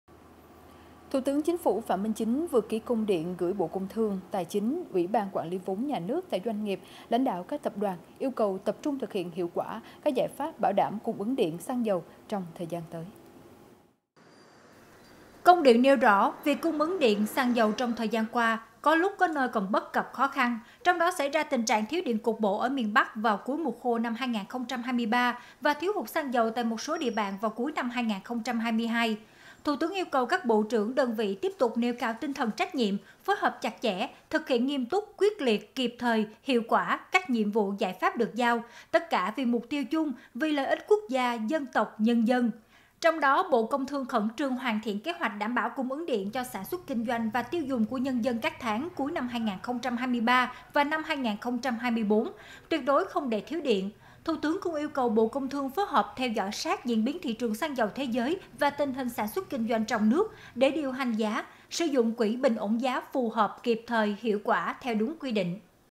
THỦ TƯỚNG YÊU CẦU KHÔNG ĐỂ THIẾU HỤT ĐIỆN, XĂNG DẦU
Thủ tướng Chính phủ Phạm Minh Chính vừa ký Công điện gửi Bộ Công thương, Tài chính; Ủy ban Quản lý vốn nhà nước tại doanh nghiệp; lãnh đạo các Tập đoàn yêu cầu tập trung thực hiện hiệu quả các giải pháp bảo đảm cung ứng điện, xăng dầu trong thời gian tới.